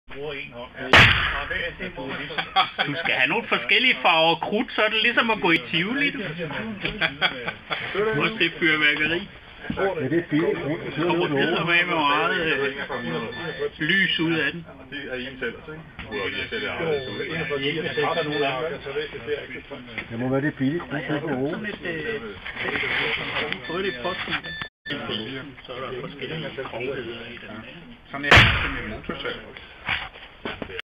Colt 36
Kalvin shooting the Colt .36